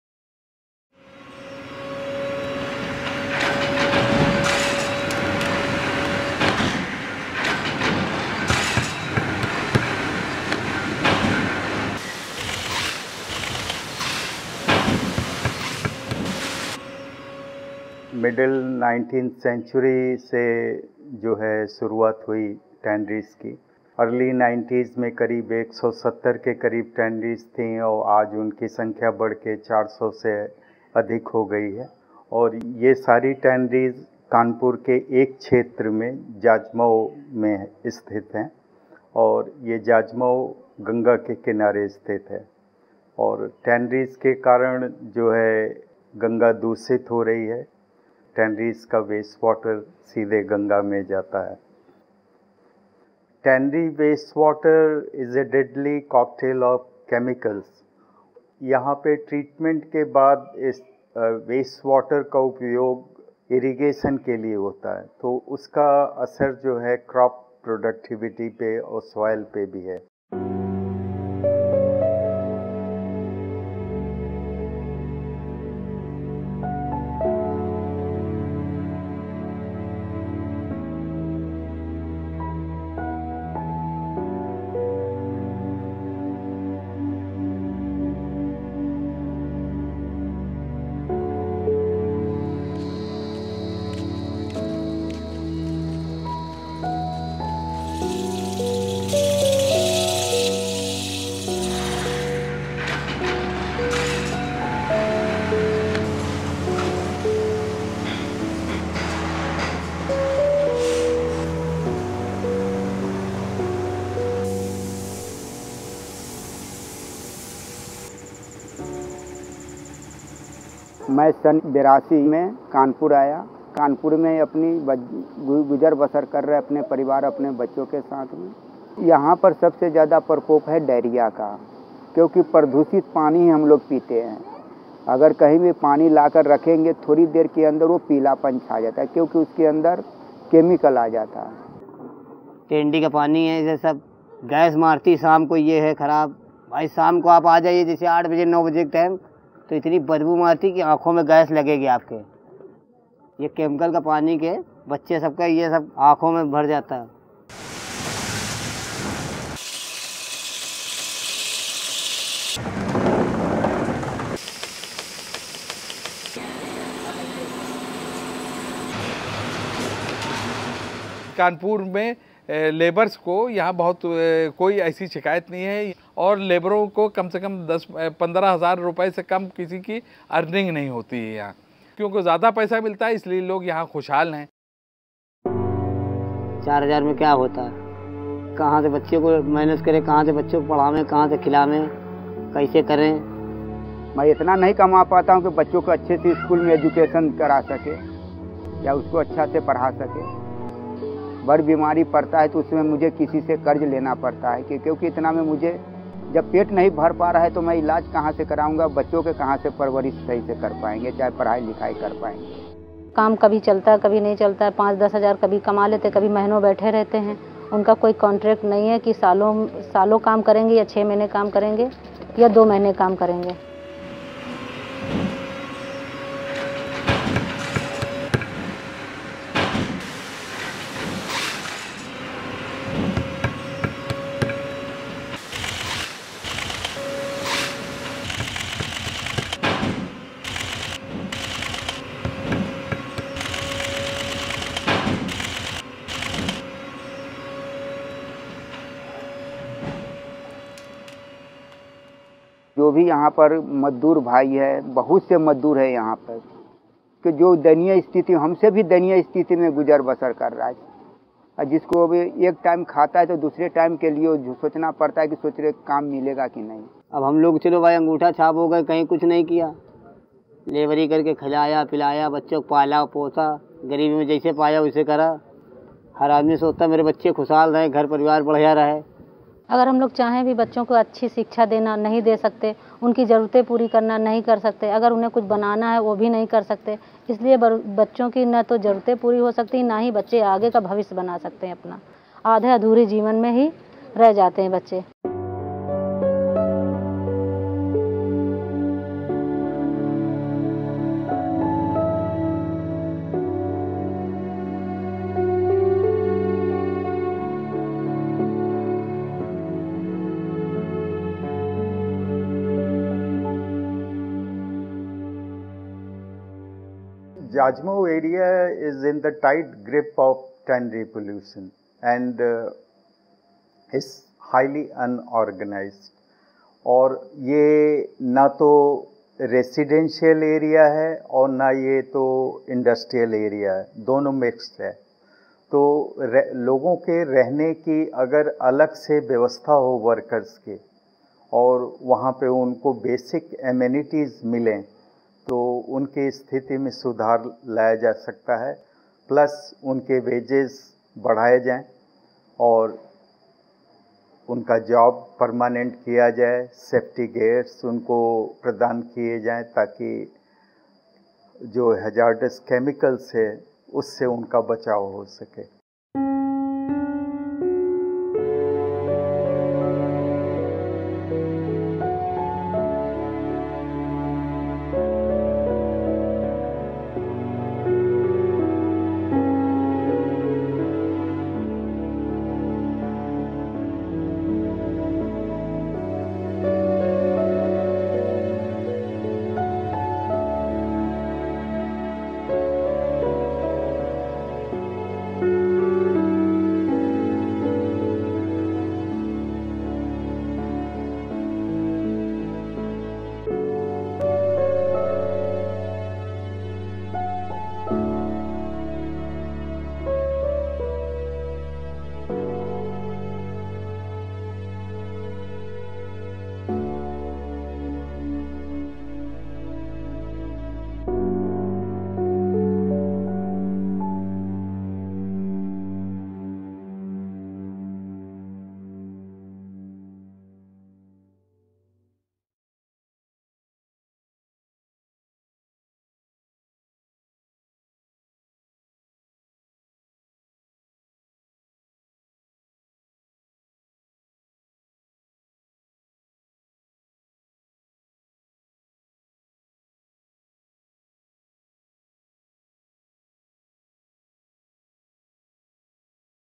Les adoberies a l'Índia
Breu documental sobre les condicions laborals de les persones que treballen a les adoberies de la indústria del calçat a l'Índia, així com de l'impacte ambiental i la contaminació a l'entorn que provoquen.
El vídeo està impulsat per la campanya Canvia de Sabates i Roba Neta.